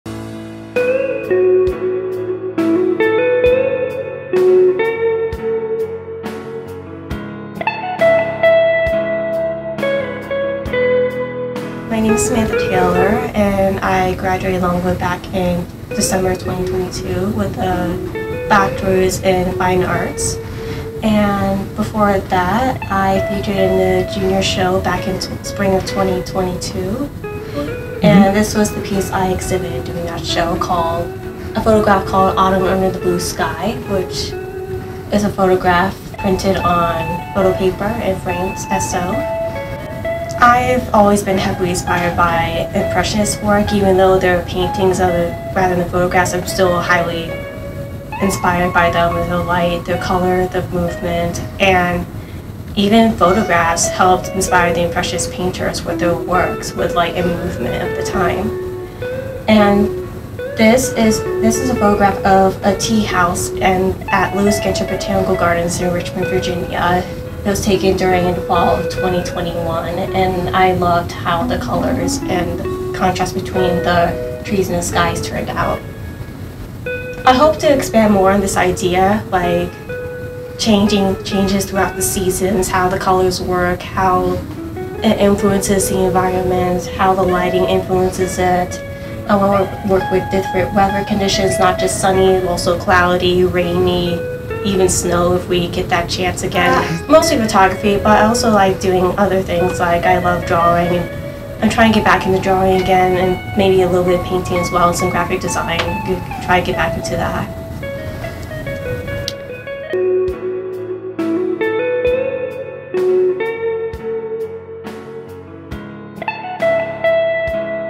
Greenwood Gallery 2022: Samantha Taylor
The Greenwood Gallery is Greenwood Library's art gallery showcasing the winners of the Greenwood Library Student Art Awards. The award recipients are awarded $200 and their artwork is displayed in the Greenwood Gallery for a year.

We're spotlighting the winners of the 2022 awards with videos describing their artwork. In this video, Samantha Taylor describes her work, "Autumn Under the Blue Sky."